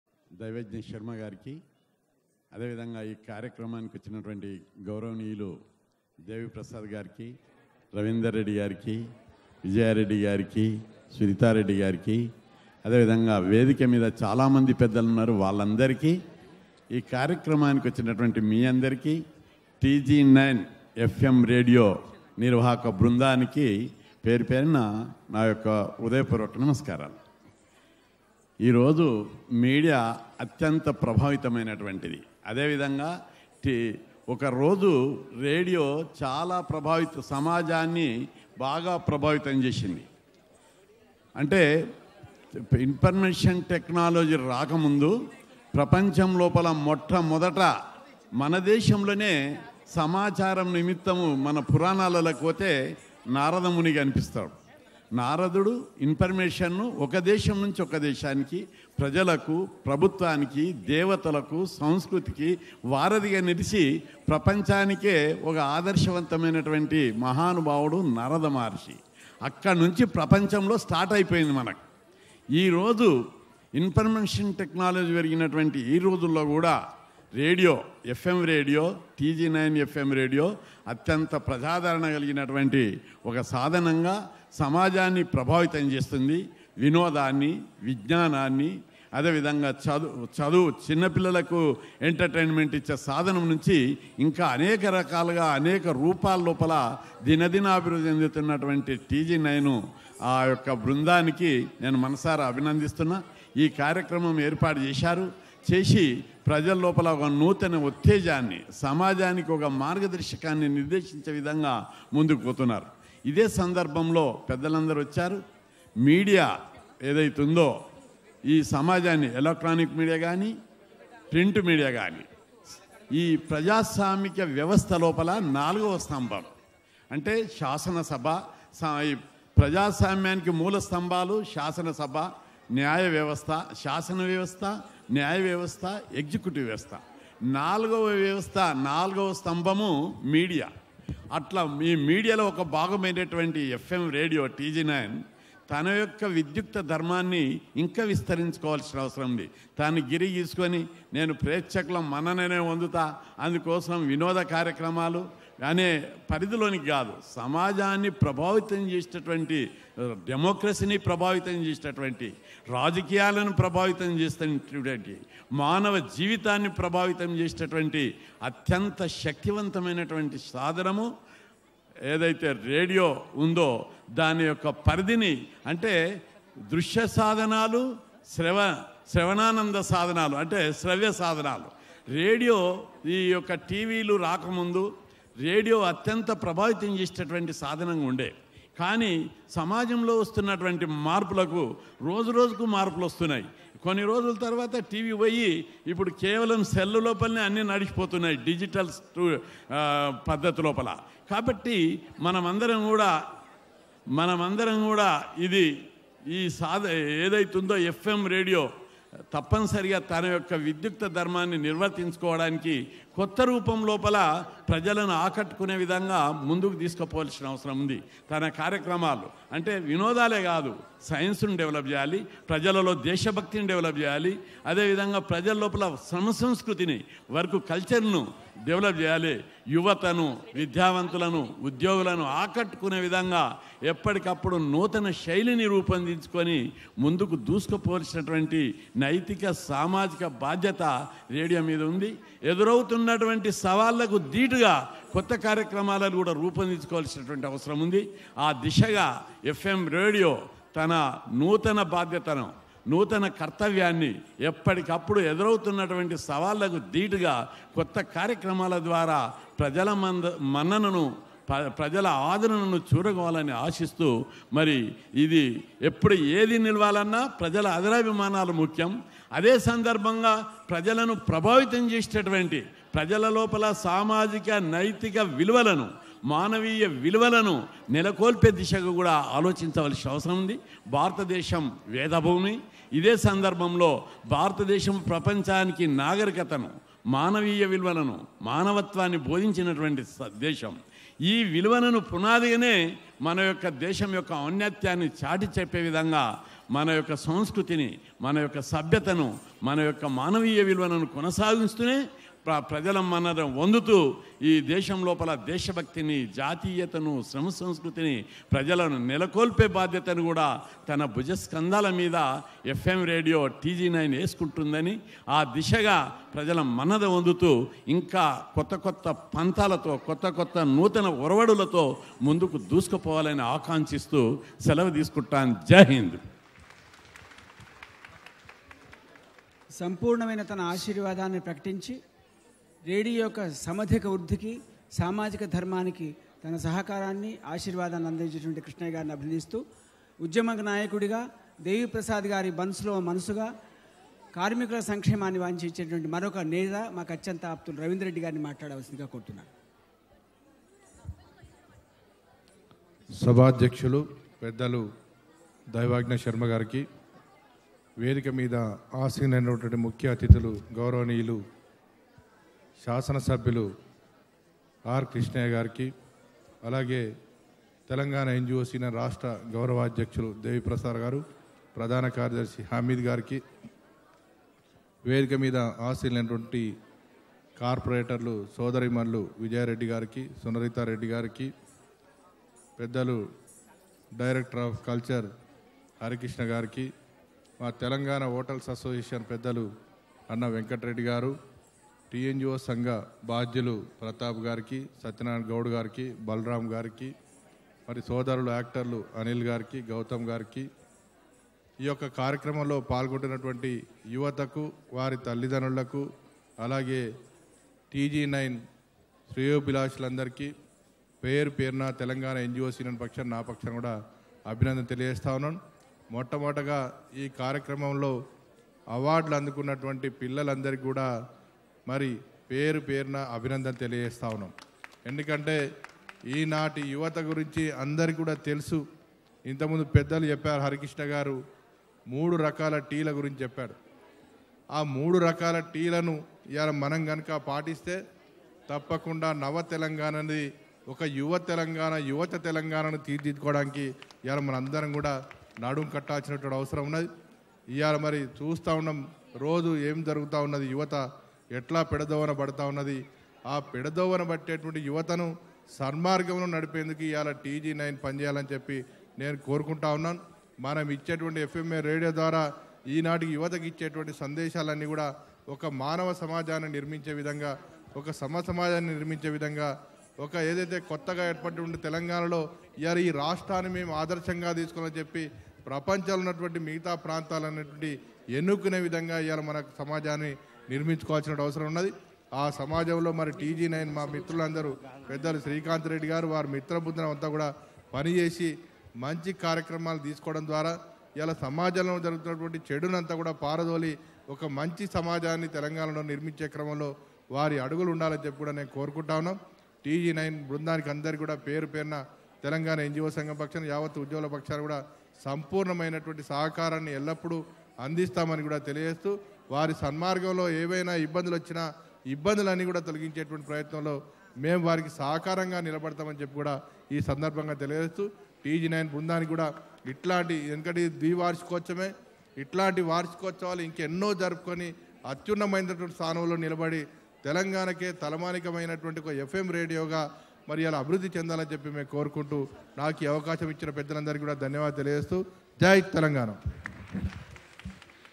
R krishnayya Speech At RJ Awards 2k16
rj awards 2k16. 1st ever rj awards in the history of online radio